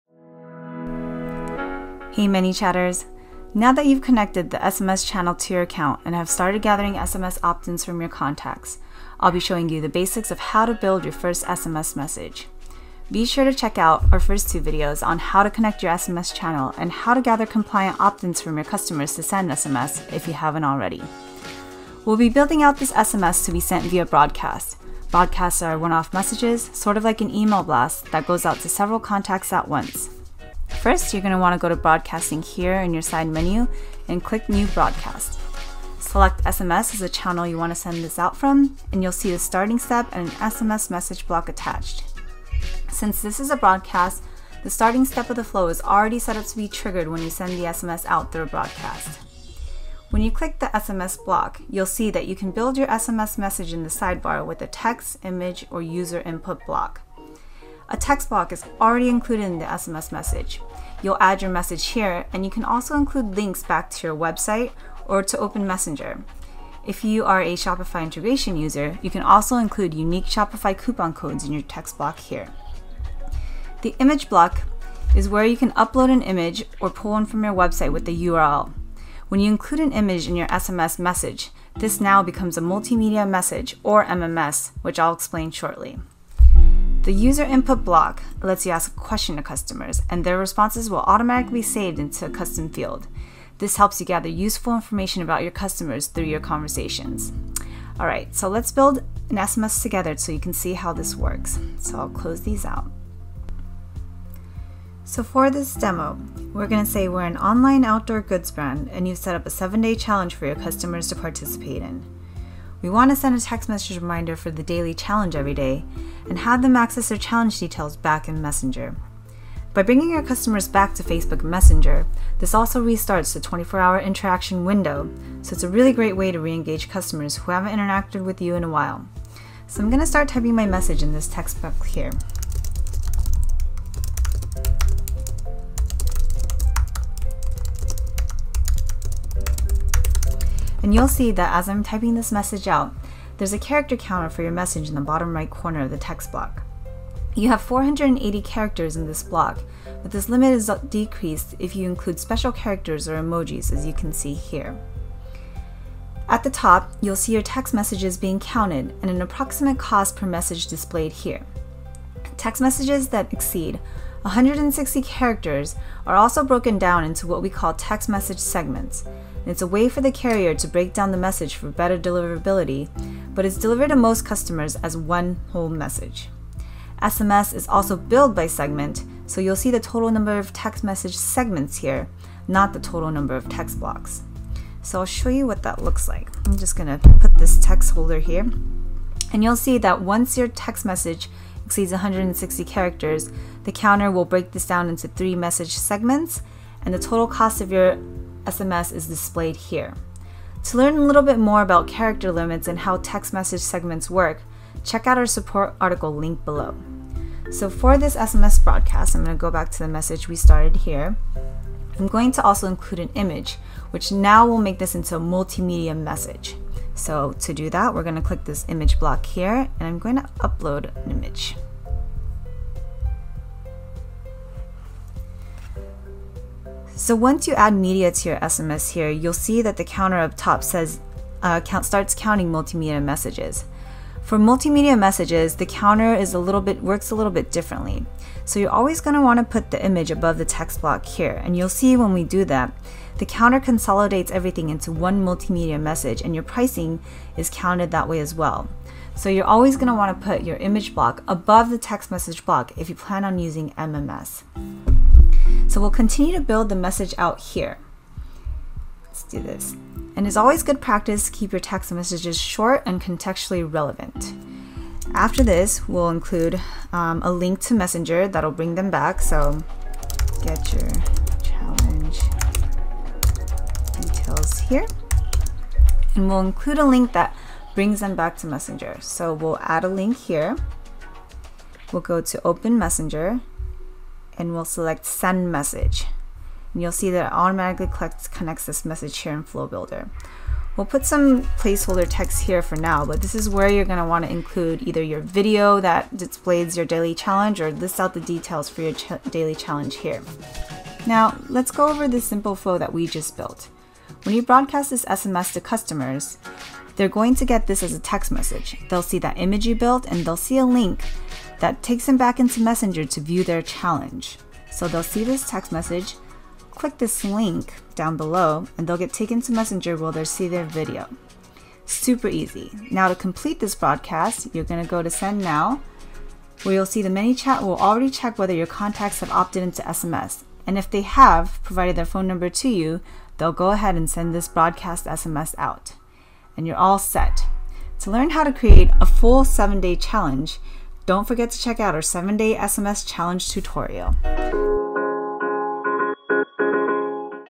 How to Build an SMS Message
Now that you know how to connect to the SMS channel and get users to opt-in, it’s time to learn how to build your SMS conversation. In this lesson, How to Build an SMS Message, follow the steps to create your first SMS Broadcast message. In addition, you’ll learn what actions to implement to re-engage with users when outside of the 24-hour window.